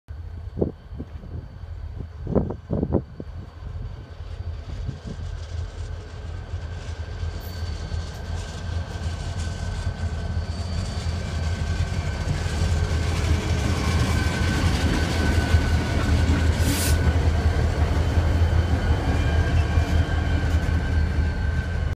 Fresh Repainted BNSF GP38-2 2641 and SD40-2 1612 Lead by 1956 SD40-2
BNSF 1956, 2641 and 1612